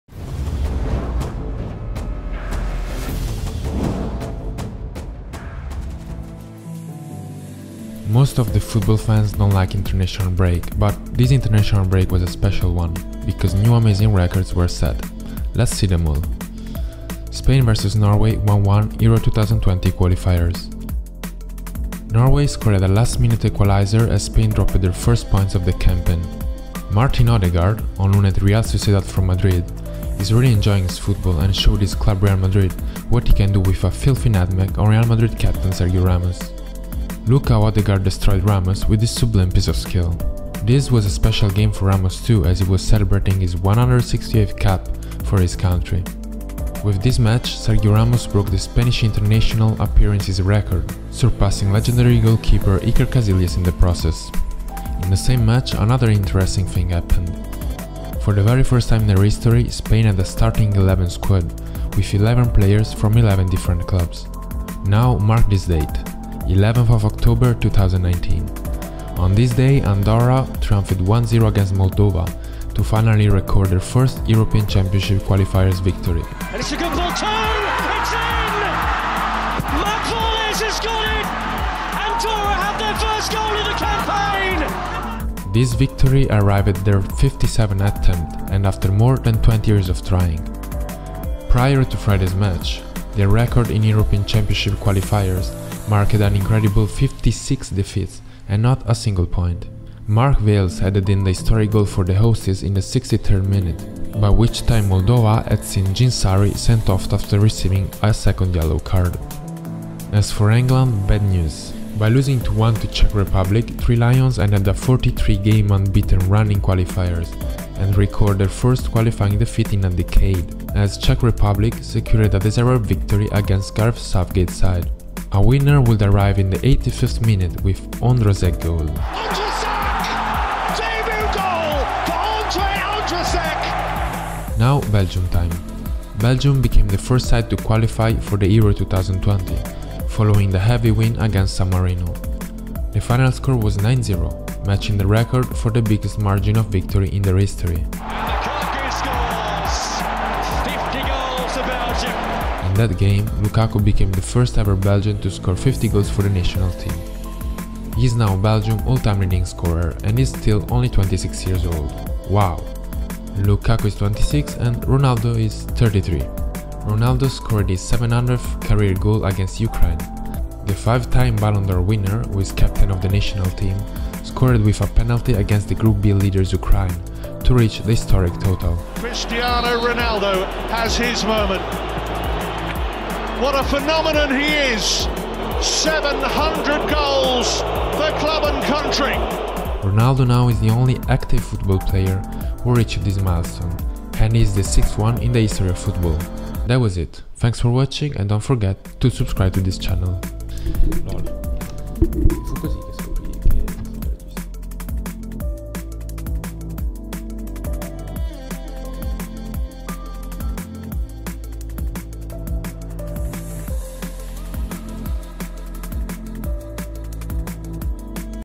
New Amazing Football Records this International Break Left Us
This international football break was a special one, with many football players and National teams accomplishing new records